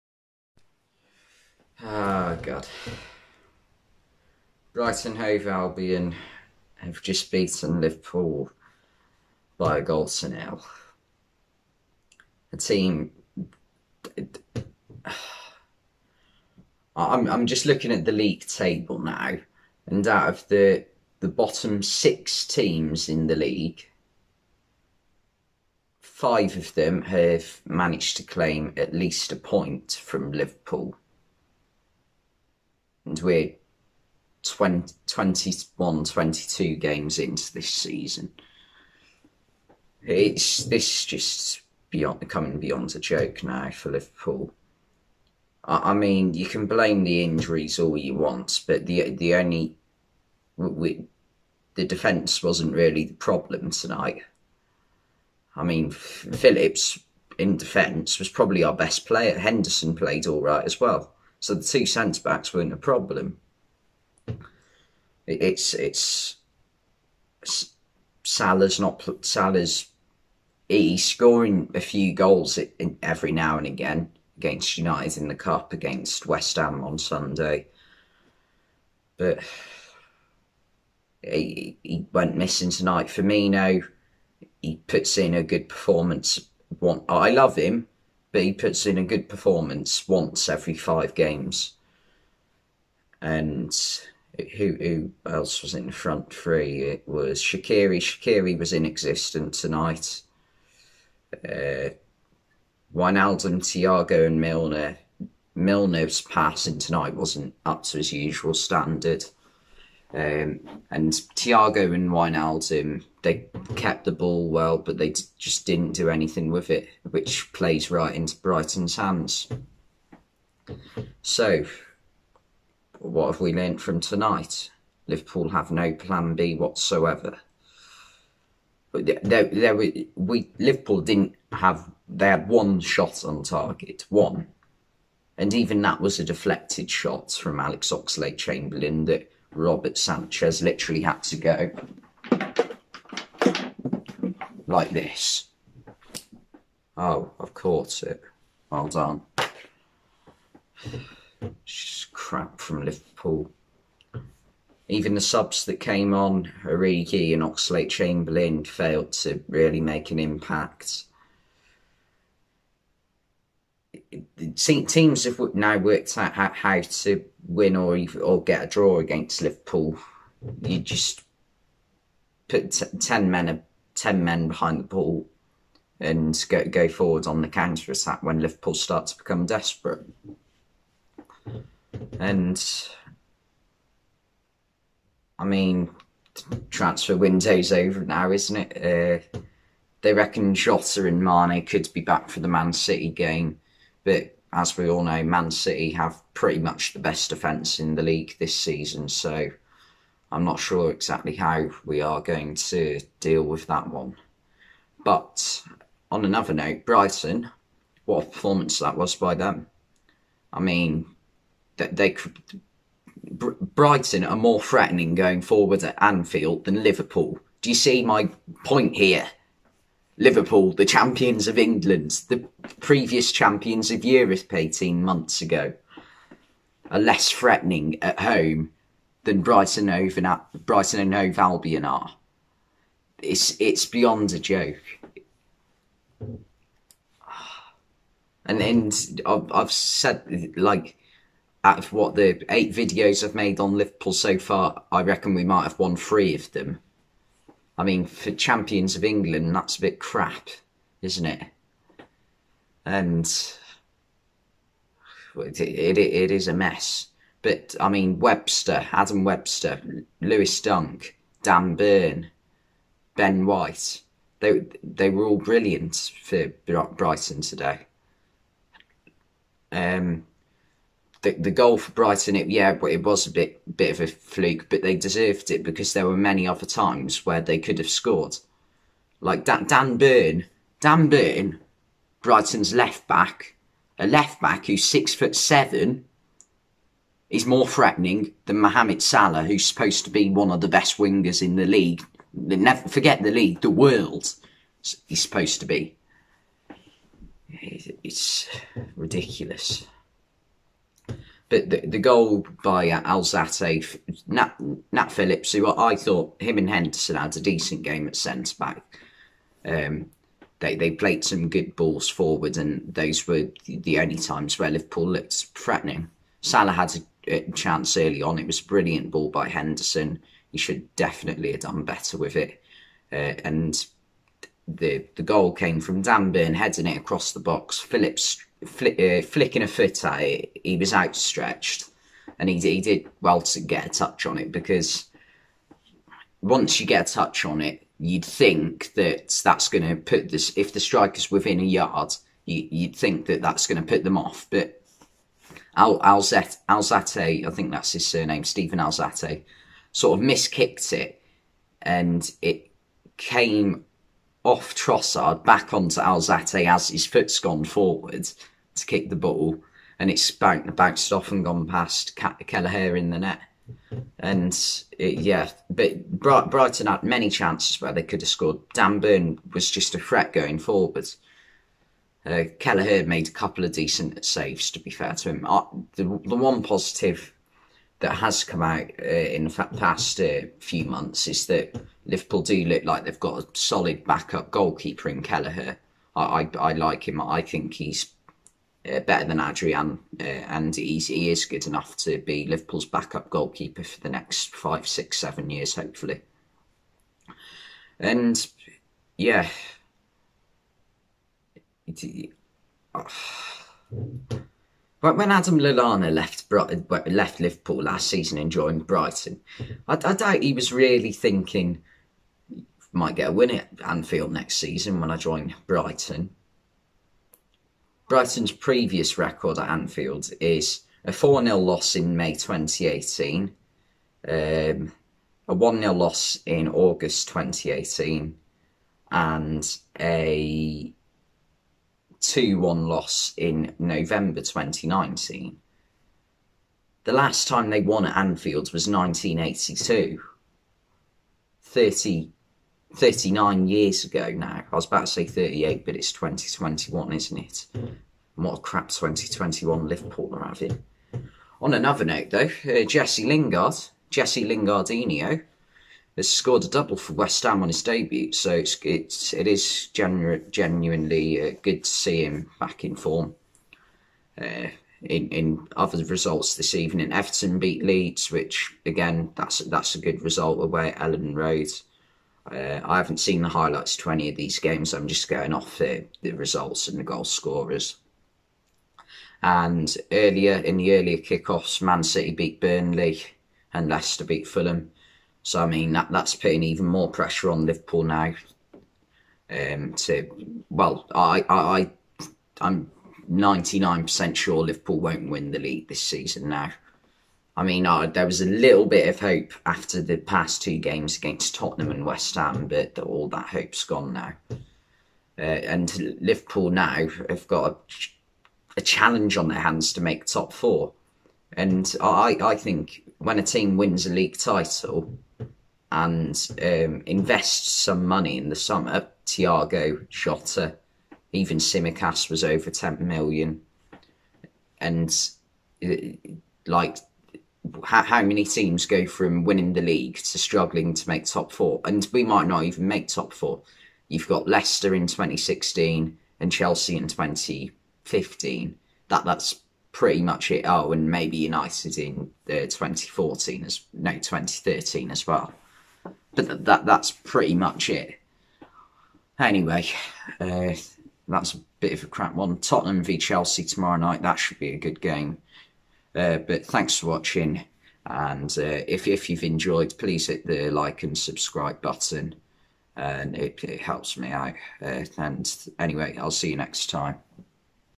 Match Reaction - Liverpool 0v1 Brighton & Hove Albion - A Pacy Summary #34
Brighton & Hove Albion have just beaten Liverpool by a goal to nil. Here are my thoughts on the game.